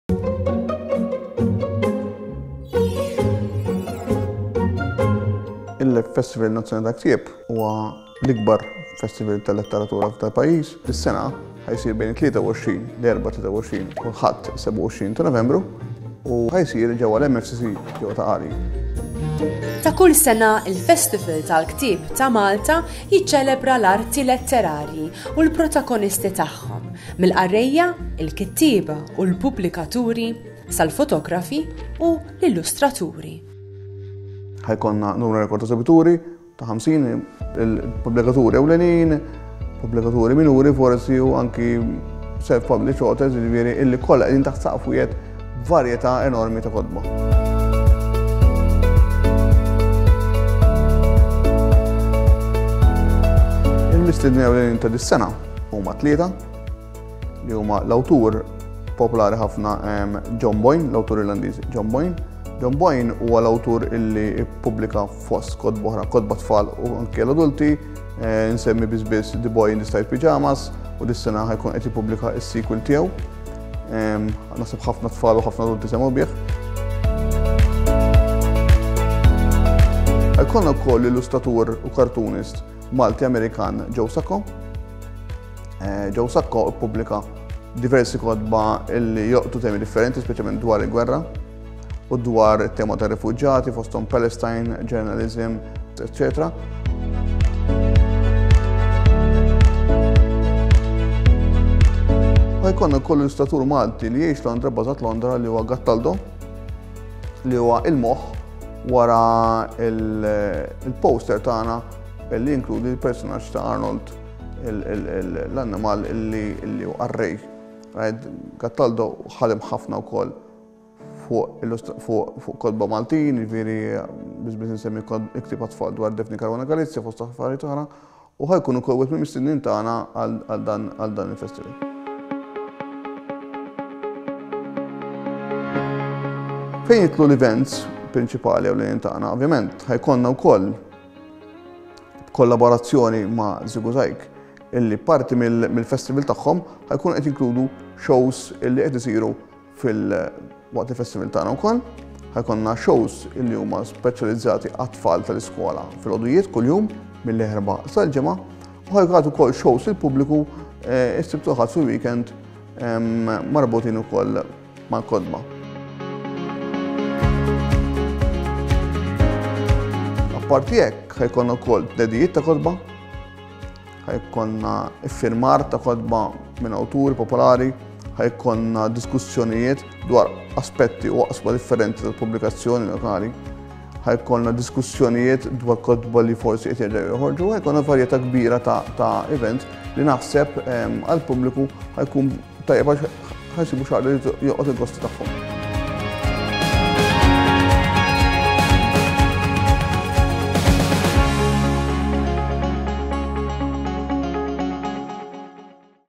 IL-FESTIVAL NAZZJONALI TAL-KTIEB
Ta’ kull sena, il-Festival tal-Ktieb ta’ Malta jiċċelebra l-arti letterarji u l-protagonisti tagħhom, mill-qarrejja, il-kittieba, u l-pubblikaturi, sal-fotografi u l-illustraturi …

F’Maltarti se nwasslulkom tagħrif dwar l-edizzjoni li jmiss tal-Festival Nazzjonali tal-Ktieb u dwar il-programm speċjali mħejji għalih …

THE MALTA BOOK FESTIVAL

Every year, the Malta Book Festival celebrates literature and its protagonists, from readers, writers, and publishers, to photographers and illustrators...

In Maltarti we’re giving you information about the next edition of the next National Book Festival and the special programme that has been set …